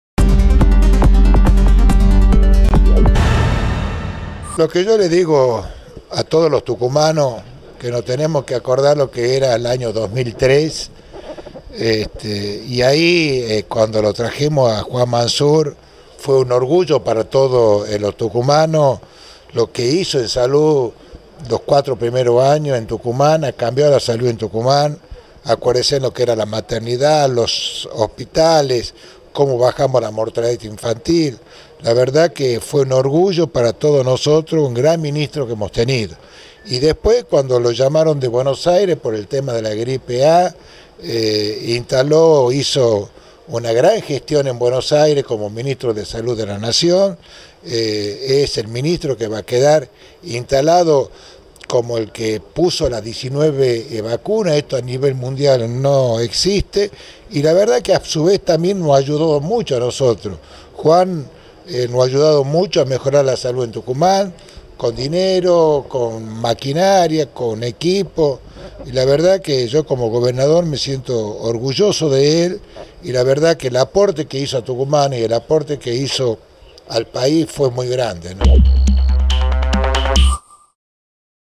“El ministro Juan Manzur cambió la salud en Tucumán”, calificó Alperovich
El gobernador José Alperovich resaltó la tarea del ministro de salud de la Nación,  Juan Manzur y remarcó los logros obtenidos en salud a nivel local y nacional.
“Tenemos que acordarnos lo que era Tucumán en 2003, cuando trajimos a Juan Manzur fue un orgullo para todos los tucumanos, lo que hizo en salud los cuatro primeros años en Tucumán, cambió la salud en Tucumán, acuérdense lo que era la maternidad, los hospitales, cómo bajamos la mortalidad infantil, fue un orgullo para nosotros, tuvimos un gran ministro”, remarcó Alperovich luego de inaugurar el Departamento Judicial de la Policía.